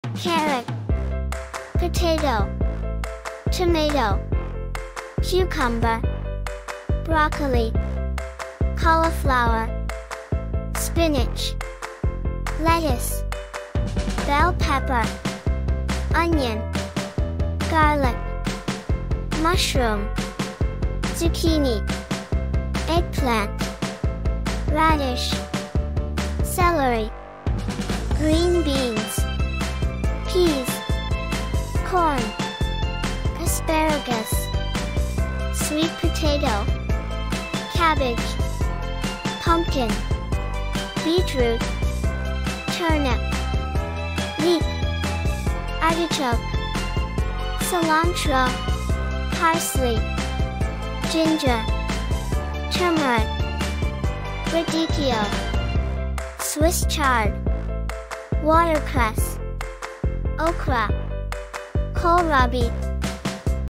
Fascinating Vegetables for Kids | Learning English and Veggie Names | Children's Education
In this video, we'll explore various common vegetables in American English. Kids will discover the names and shapes of familiar veggies like carrots, potatoes, tomatoes, cucumbers, and many more. This is a fantastic way to help children expand their vocabulary and develop a love for eating vegetables!

.Social .

.Hastag.